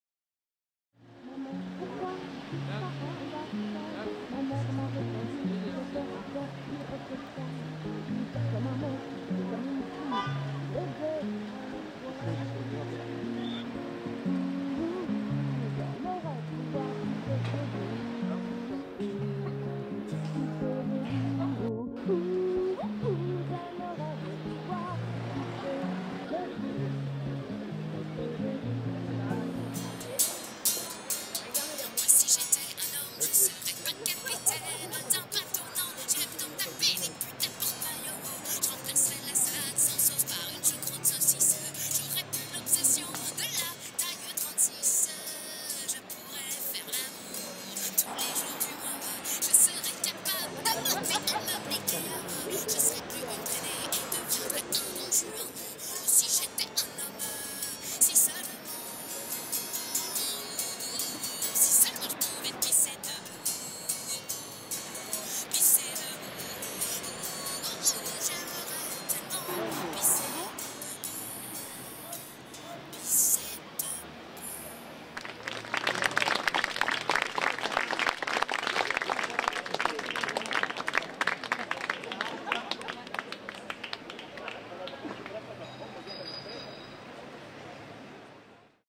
GiedRé @ SeXodRoMe
LiVe caSQué @ seXodRoMe daNs Le cadRe du MaMA FeSTiVaL - 21 oCT 2011